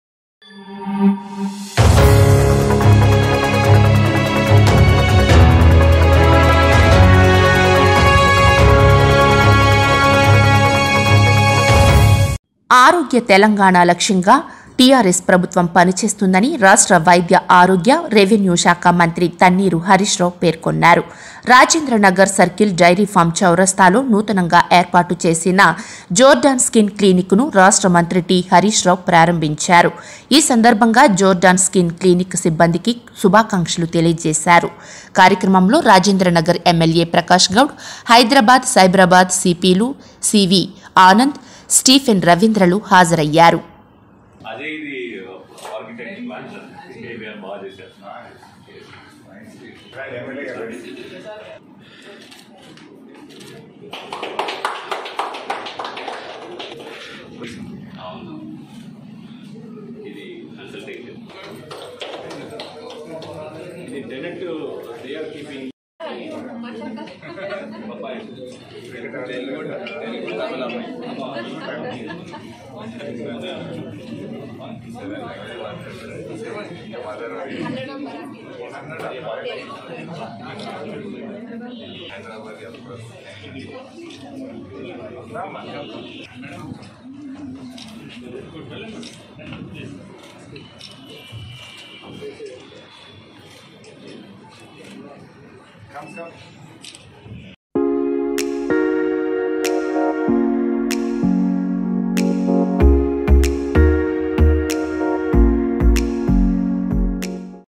డైరీ ఫామ్ చౌరస్తాలో నూతనంగా ఏర్పాటు చేసిన JORDON CLINIC ను రాష్ట్రమంత్రి టి.హరీష్ రావు ప్రారంభించారు
డైరీ ఫామ్ చౌరస్తాలో నూతనంగా ఏర్పాటు చేసిన JORDON CLINIC ను రాష్ట్రమంత్రి టి.హరీష్ రావు ప్రారంభించారు|| Y9NEWS TELUGU